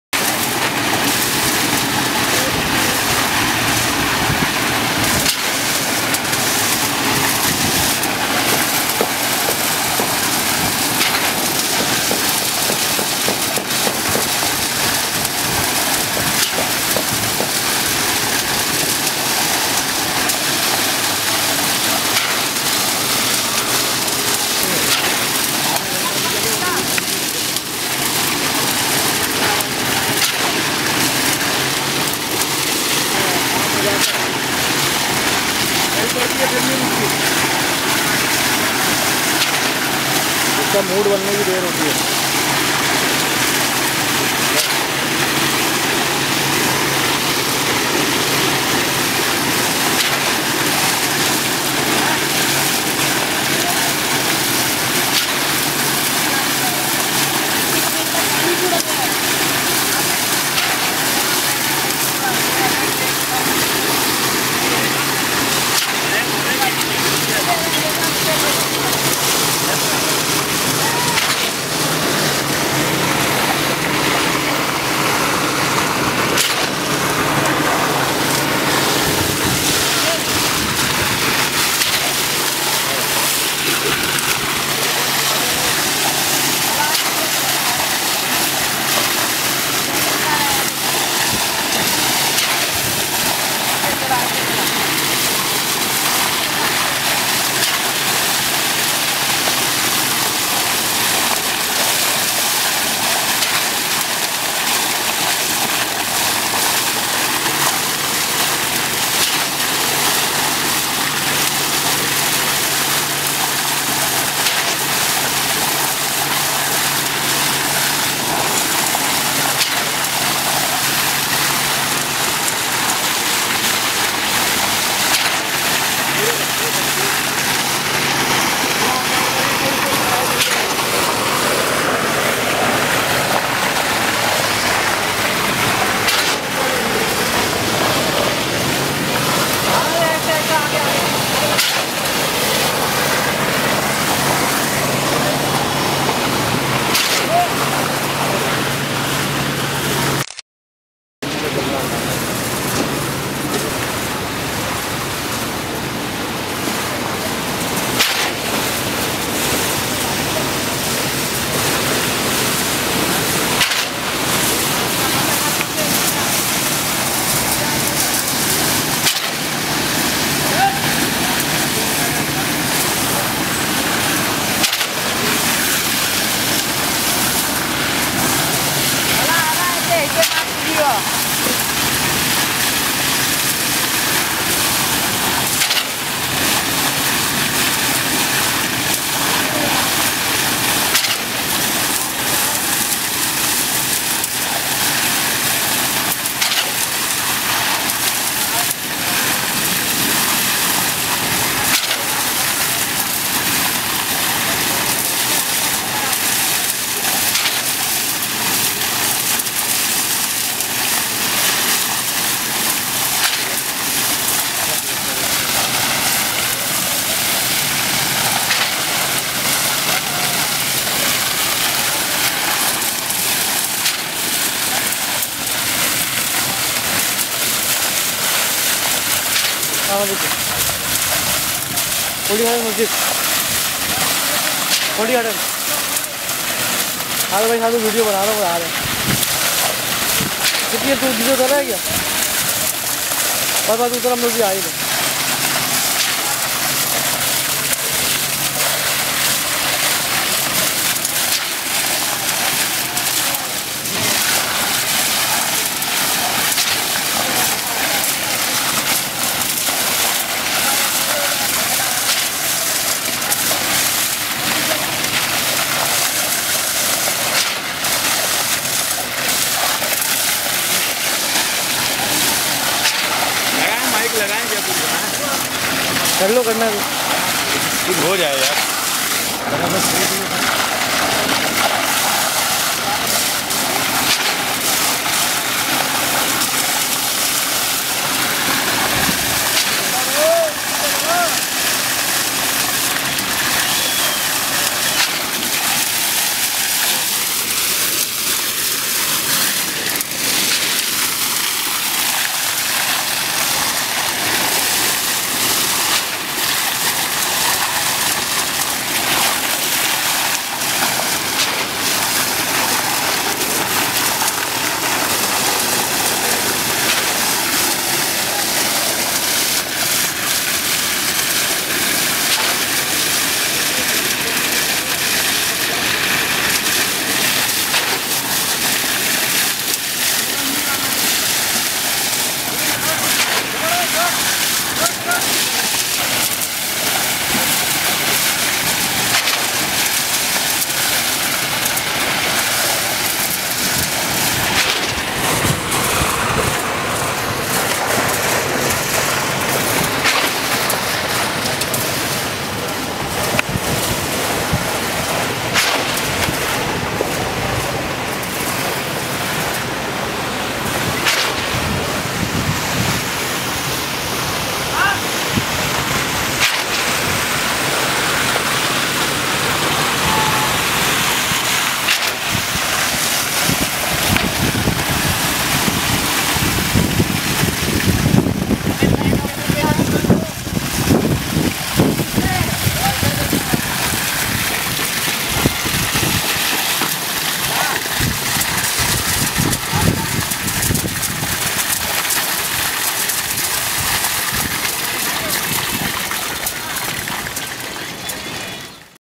Memon Masjid Muslehuddin Garden | Roof ki Bharai | Part-2
میمن مسجد مصلح الدین گارڈن کی تعمیر نو کے مناظر

Muhammad Shahrukh Qadri